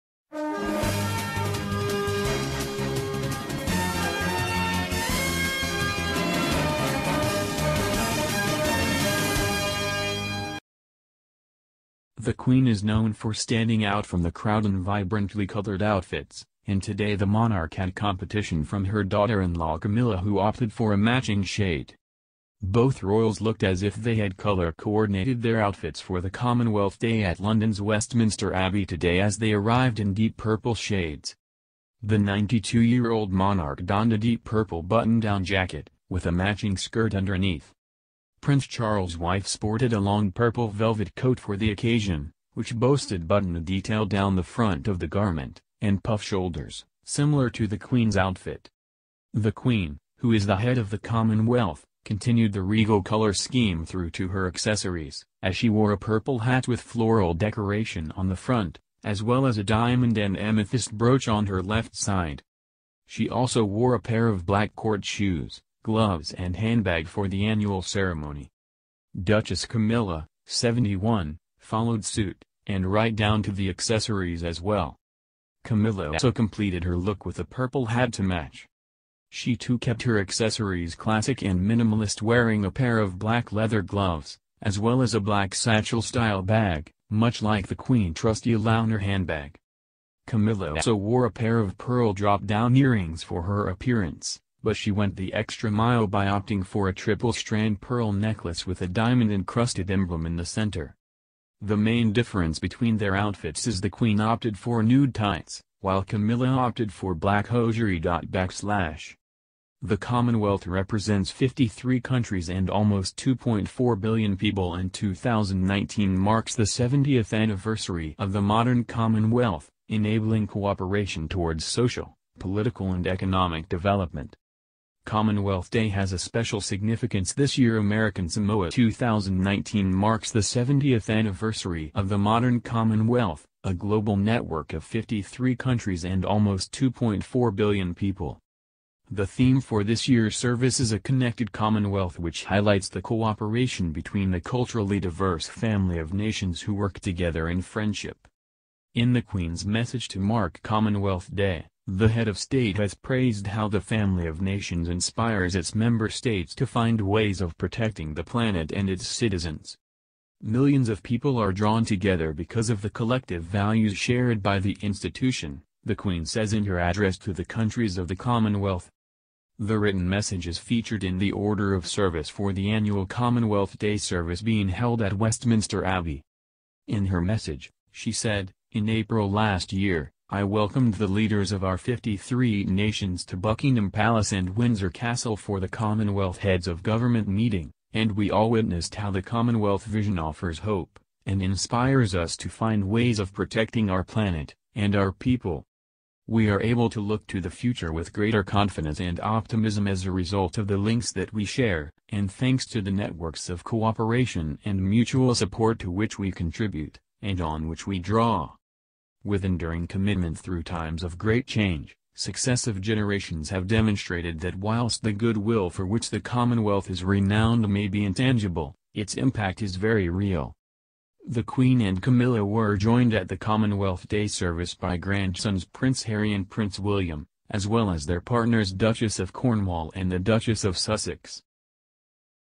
Queen and Camilla don matching shades as they join senior royals for the Commonwealth Day service
Purple reign! Queen and Camilla don coats and hats in matching shades of plum as they attend Commonwealth Day service at Westminster Abbey